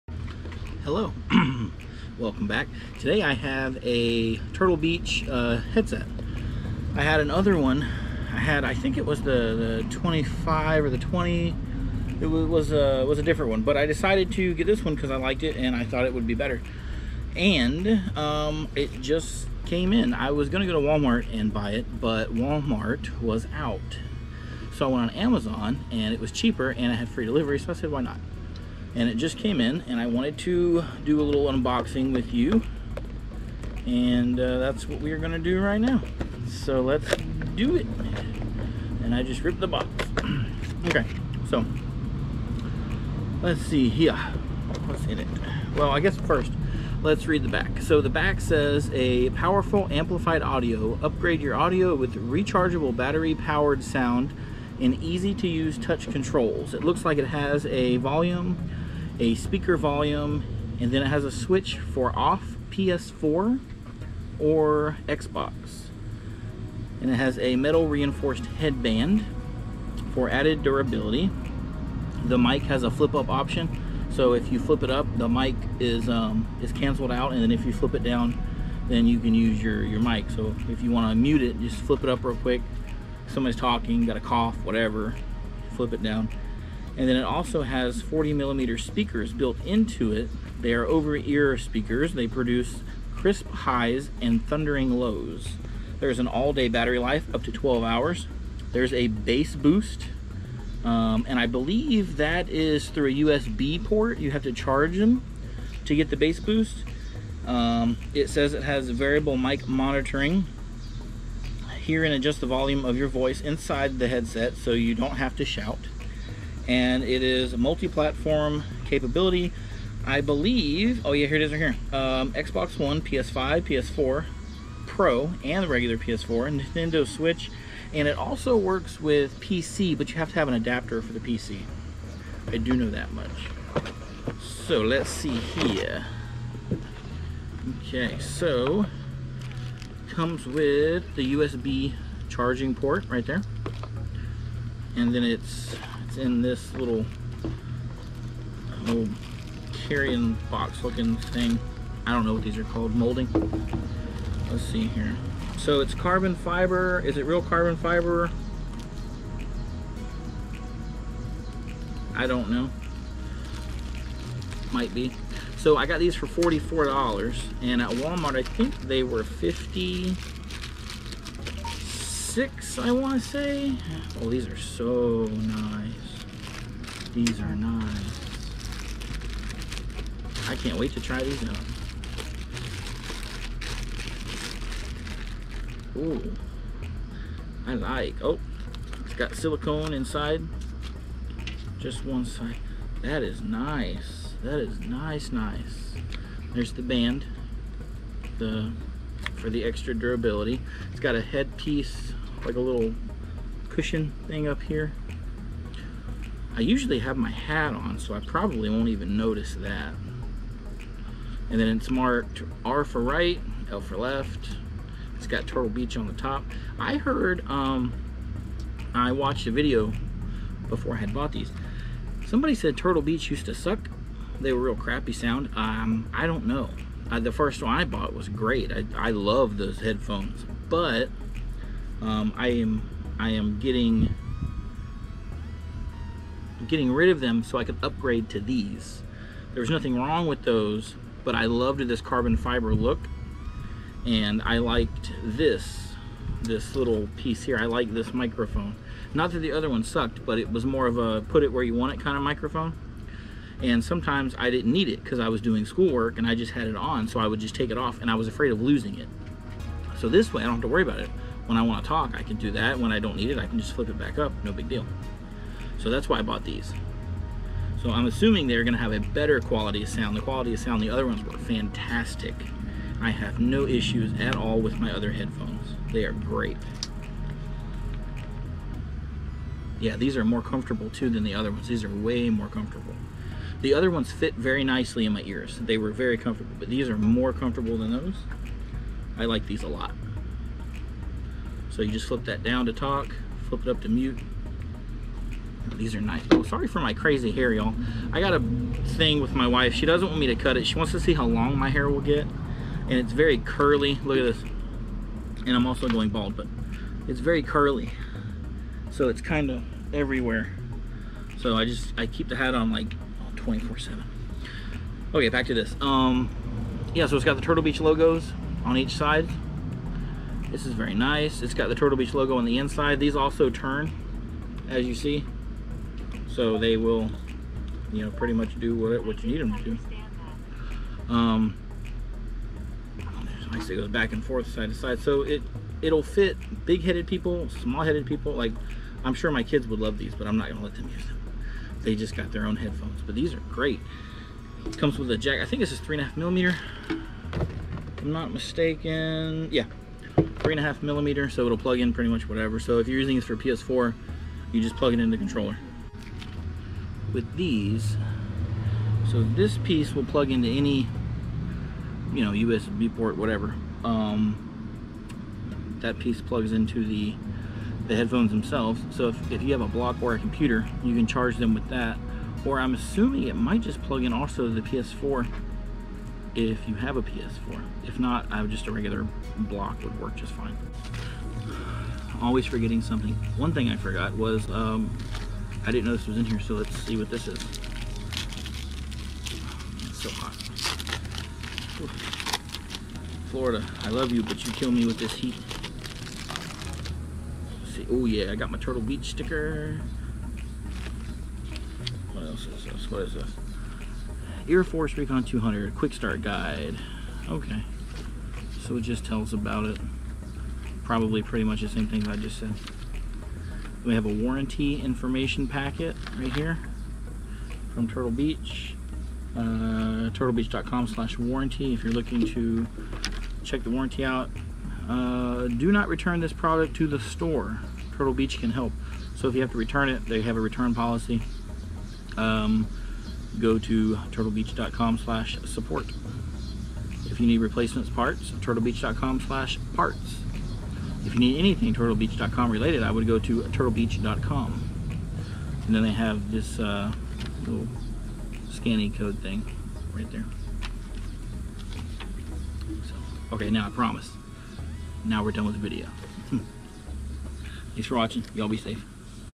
Turtle Beach Recon 200 Carbon Fiber Headset
Welcome back to another unboxing video! Today we have the Turtle Beach Recon 200 in Carbon Fiber. I have set of the Turtle Beach 50P headphones and I love them but I seen these with the carbon fiber and the different speaker set up and wanted to see what there were like. I hope this video helps.